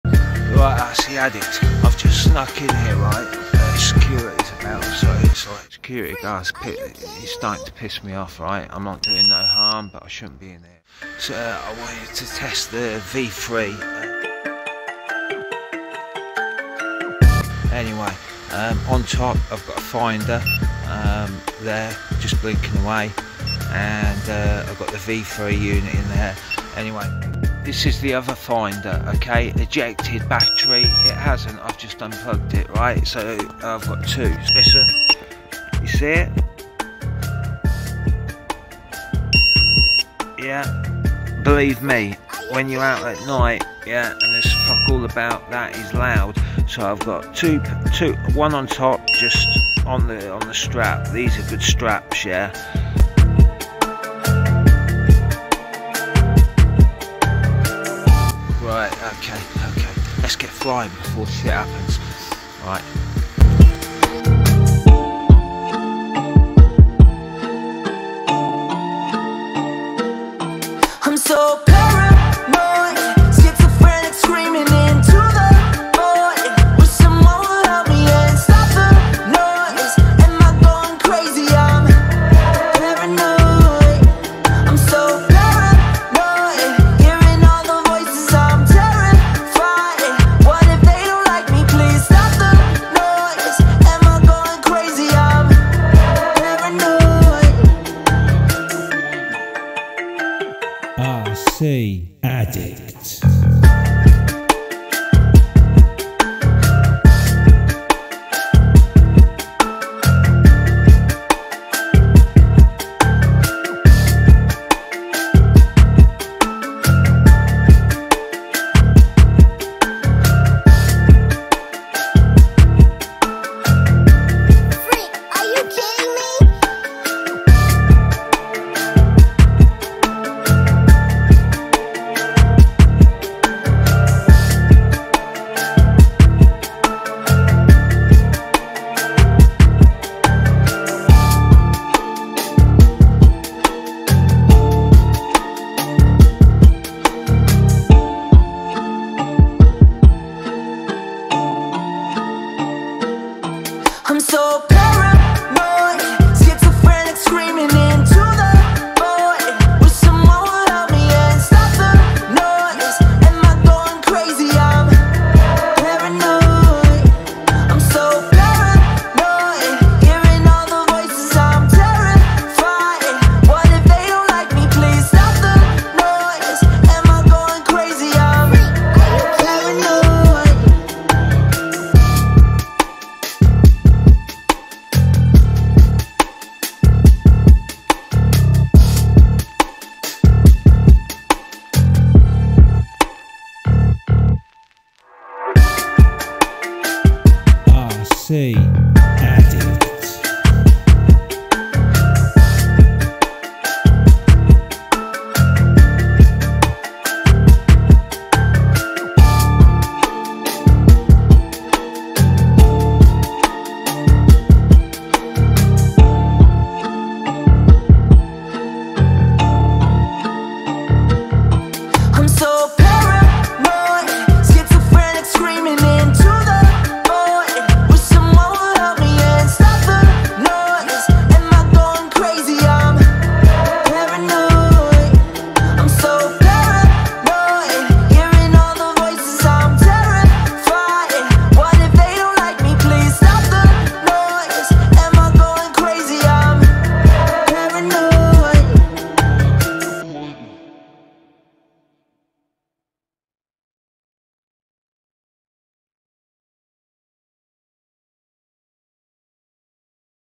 SMASHING WALKSNAIL HD FPV V3 AT NIGHT PARANOID
WALKSNAIL V3 KIT IN 3.5" geprc ST35 FRAME KIT
walksnail V3

SHOPS IN UK FOR fpvdrone STUFF

(im so jealous!)

,,,,,,,,,,,,,,,,,,,, RTL, returntohome Full rcaddict walkthrough 4.4.

$$$FULL SETTINGS! WALKTHROUGH!$$$$

RESPECT TO ALL!
HAVE A SAFE XMAS